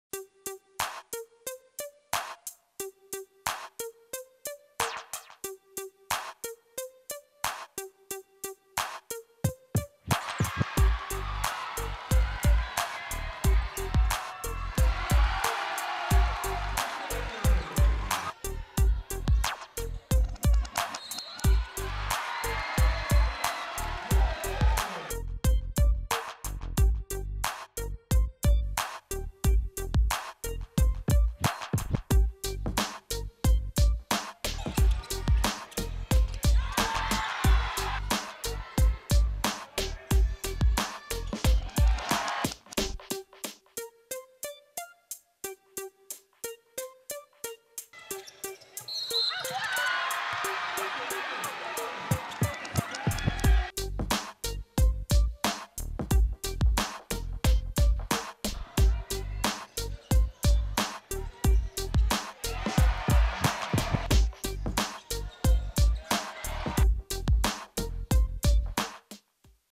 FAU highlights vs FIU WBBALL
The Owls took down rival FIU for the first sweep since 2016. The FAU women's basketball team has improved to 6-8 in conference play, 12-13 overall.